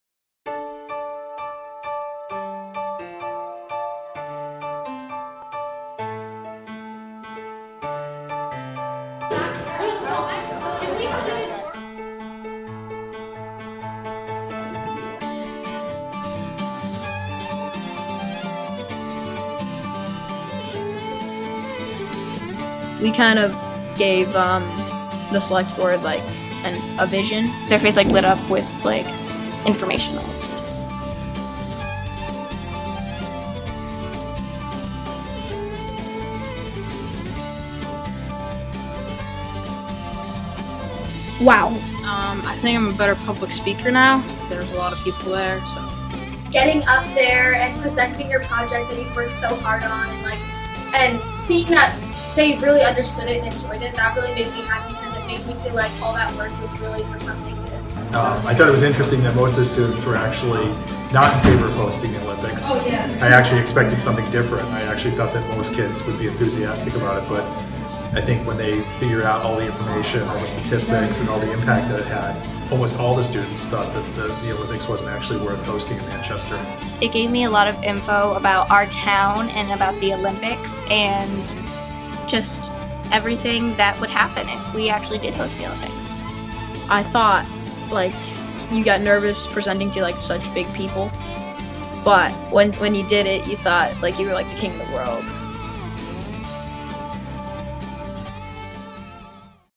Should Vermont host the next Olympics?
6th graders in the town of Manchester, VT had the chance to advise the town's Selectboard on whether Manchester should put in a bid to host the next Olympics. The students conducted research and interviews all to help Manchester with this very big decision...